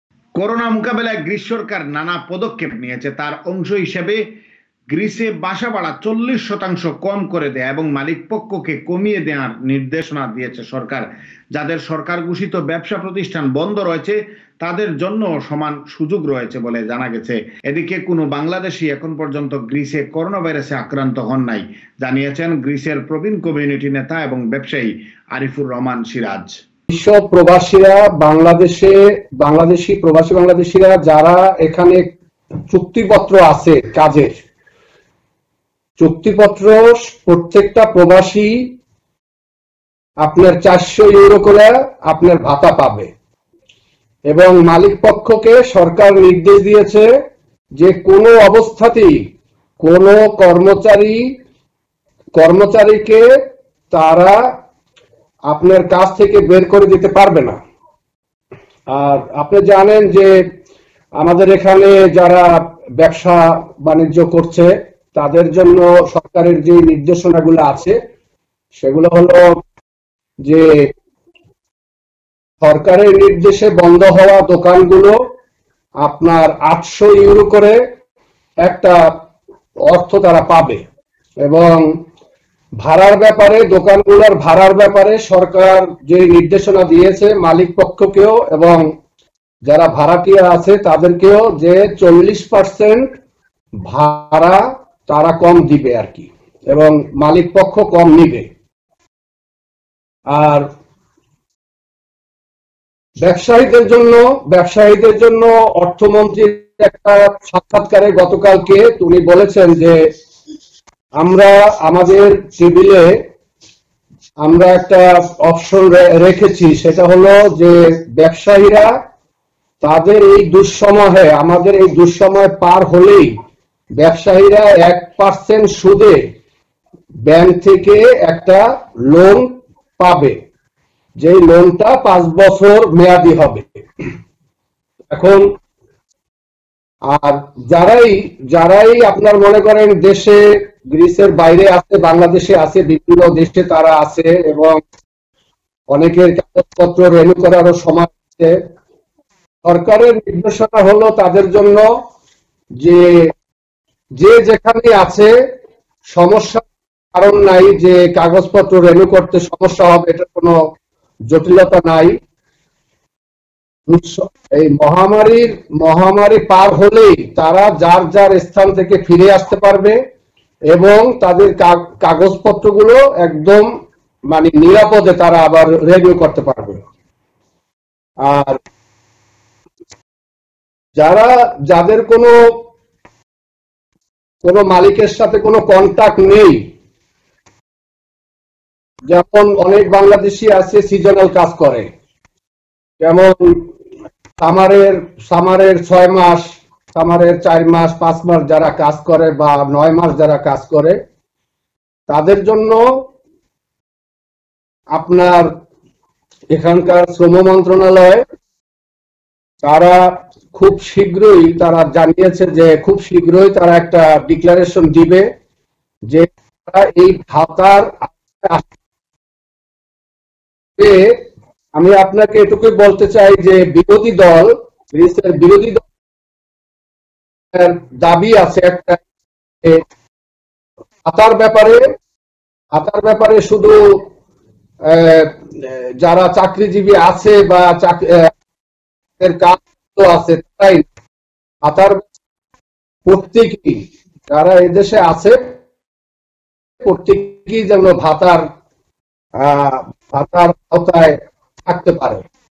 গ্রিসে বাসাভাড়া চল্লিশ শতাংশ কম করে দেয়া এবং মালিক পক্ষকে কমিয়ে নেয়ার নির্দেশনা দিয়েছে সরকার
গ্রিসে বাসাভাড়া  চল্লিশ শতাংশ কম করে দেয়া এবং মালিক পক্ষকে কমিয়ে নেয়ার নির্দেশনা দিয়েছে সরকার  ।যাদের সরকার ঘোষিত ব্যবসা প্রতিষ্ঠান বন্ধ রয়েছে তাদের জন্যও সমান সুযোগ রয়েছে।
এদিকে কোন বাংলাদেশী গ্রিসে করোনা আক্রান্ত হয় নাই জানিয়েছেন
প্রবীণ কমিউনিটি নেতা আরিফুর রহমান সিরাজ